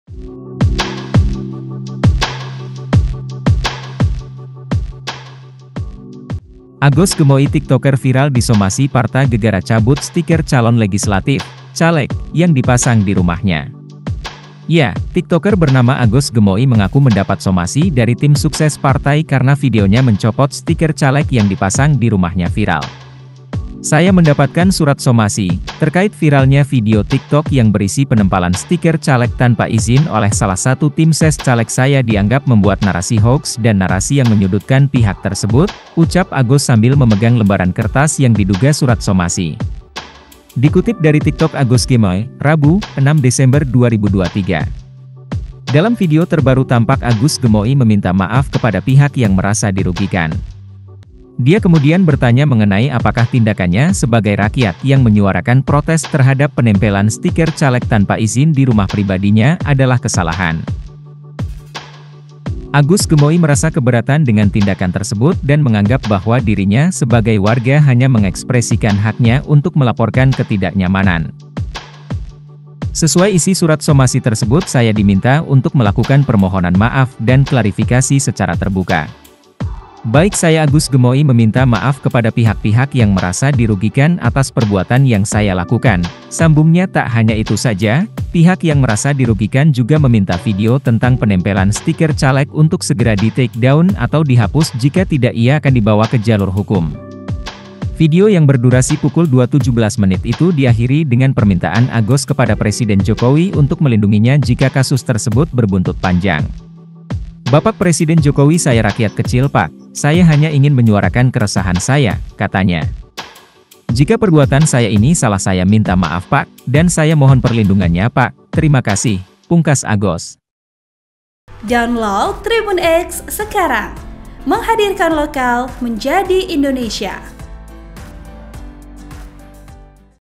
Agos Gemoy Minta Tolong Jokowi Setelah Disomasi Tim Sukses Partai
Agos Gemoy Tiktoker viral disomasi parta gegara cabut stiker calon legislatif (caleg) yang dipasang di rumahnya.

Ya, TikToker bernama Agos Gemoy mengaku mendapat somasi dari tim sukses partai karena videonya mencopot stiker caleg yang dipasang di rumahnya viral.

"Saya mendapatkan surat somasi, terkait viralnya video Tiktok yang berisi penempalan stiker caleg tanpa izin oleh salah satu Timses Caleg.

VP: Darwin